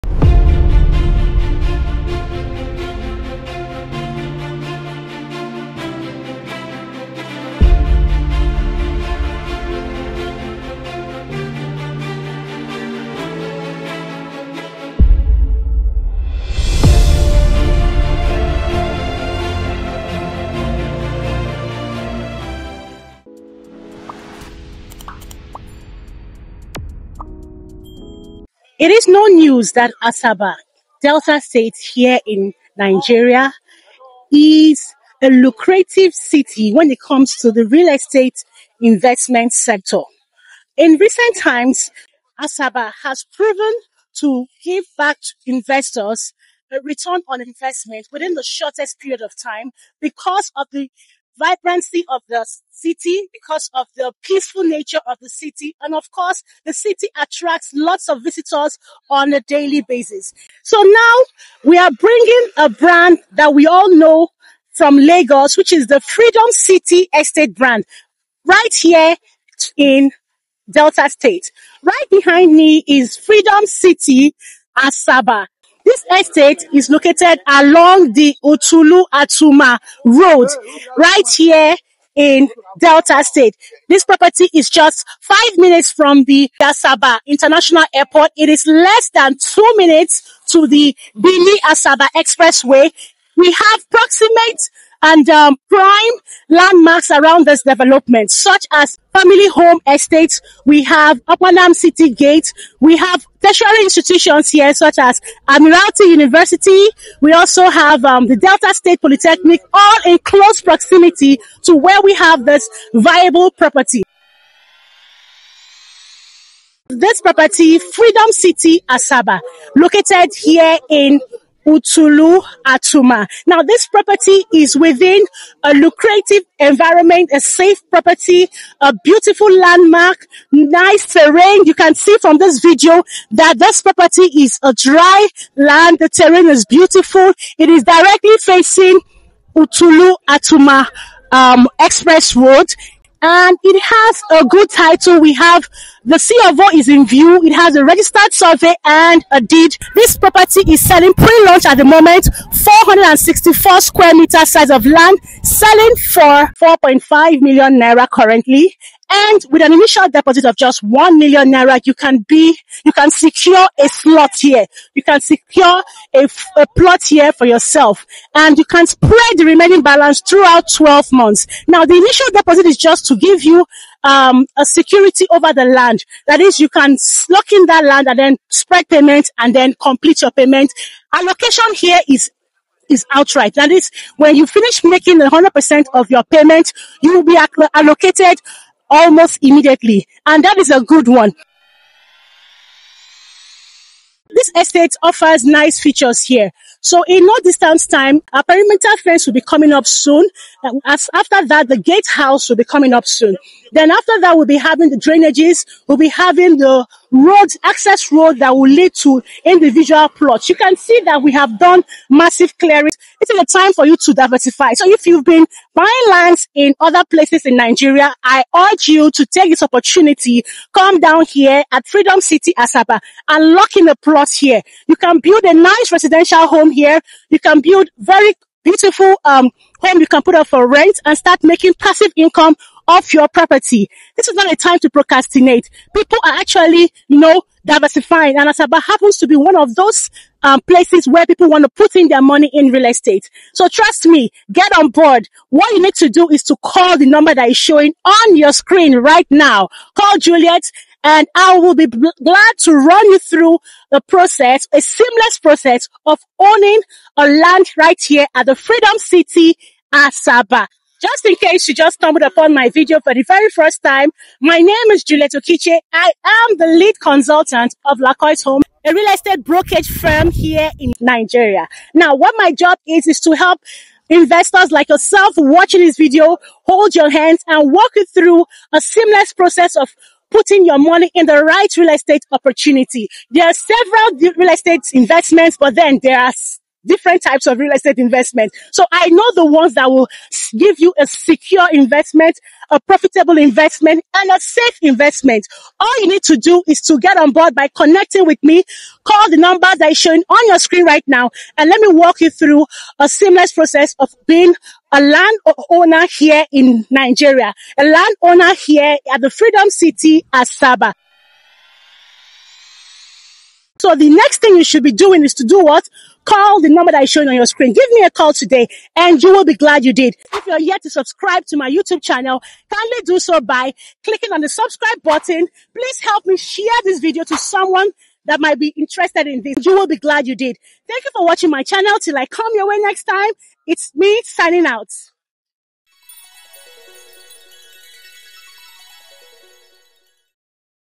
ASABA LAND FOR SALE, FREEDOM CITY ESTATE‼️‼️‼️
NEWLY LAUNCHED, FREEDOM CITY ESTATE, ASABA‼️‼️‼️

It was a successful launch of Freedom City Estate in Asaba!!!

This estate offers sales of serviced plots, within a strategically situated community, that is safe for investment!!!

Freedom City Estate, Asaba is still selling at prelaunch price, with genuine land title, it comes with an all inclusive price, instant allocation and a 6 months interest free payment plan!!!

*LOCATION:*
Akwu Kwu Igbo-Otulu, Atuma, Asaba, Delta State.

*PLOT SIZE:* 464 SQM

*LAND PRICE:*
*Pre-Launch price:* N4,500,000
*Actual Price:* N5,500,000

*INITIAL DEPOSIT:* N1,000,000

*PROXIMATE LANDMARKS*
Summing Electrical Nigeria Limited
Wichtech Industries Limited
Federal Housing Estate
Admiralty University
Delta State Polytechnic
Asaba international Airport
Family Home Estate
Okpanam City Gate
Shoprite Asaba

*TITLE*: Registered Survey | Deed of Assignment